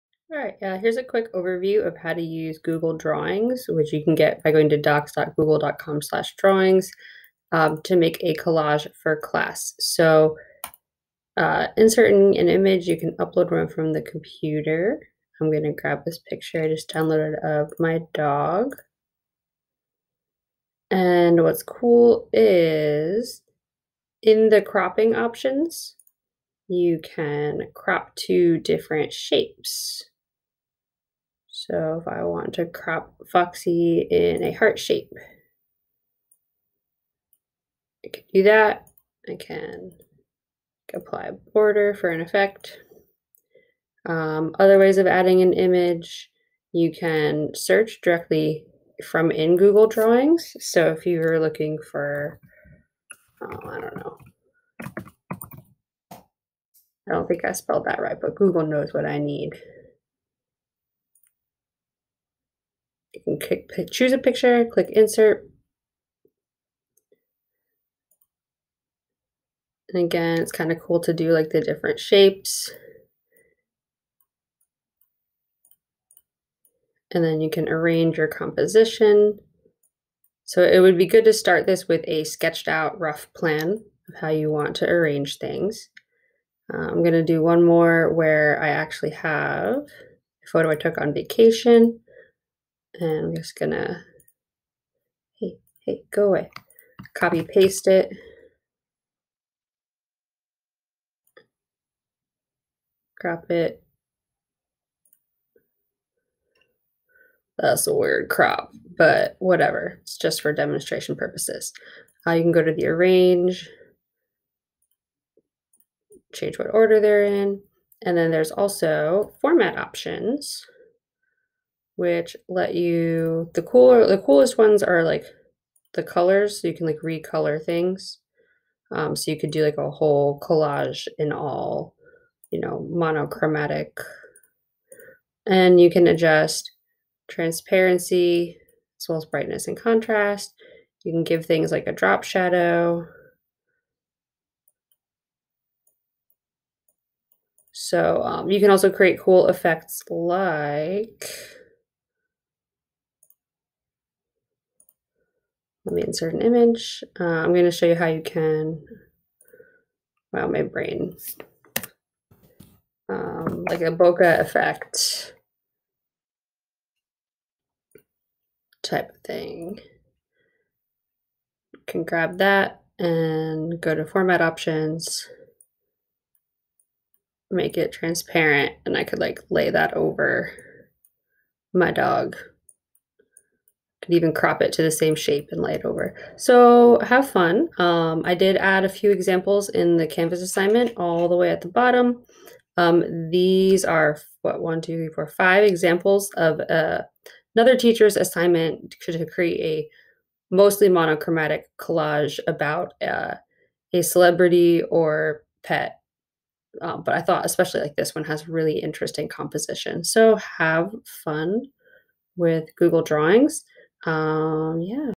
Basics of adding images, Cropping Masks, Format Options in Google Drawings
Made to show some of my students an option for a project.